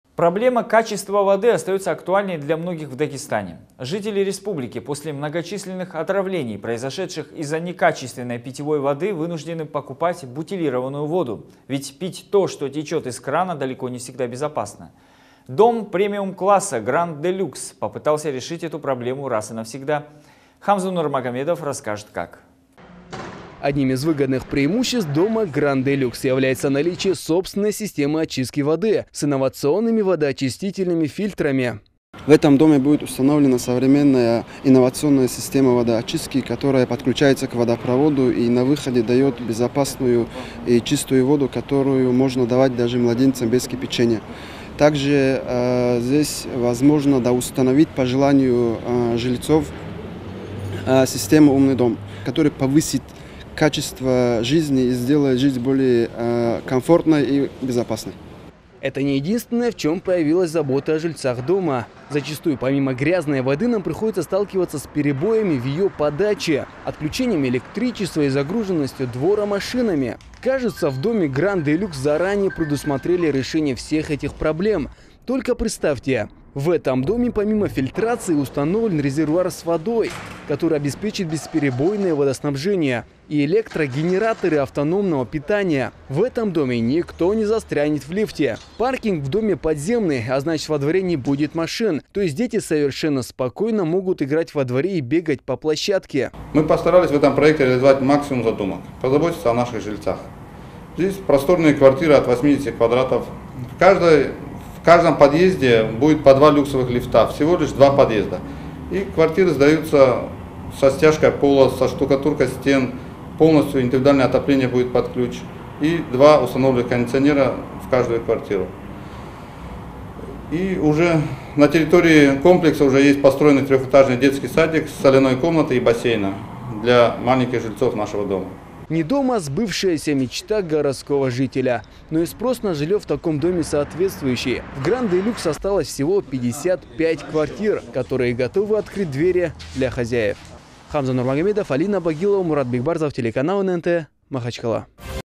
Дом мечты
Проблема качества воды остается актуальной для многих в Дагестане.

Жители республики после многочисленных отравлений произошедших из-за некачественной питьевой воды, вынуждены покупать бутилированную воду, ведь пить то, что течет из крана далеко не всегда безопасно.

Дом премиум класса «Гранд де люкс» попытался решить эту проблему раз и навсегда.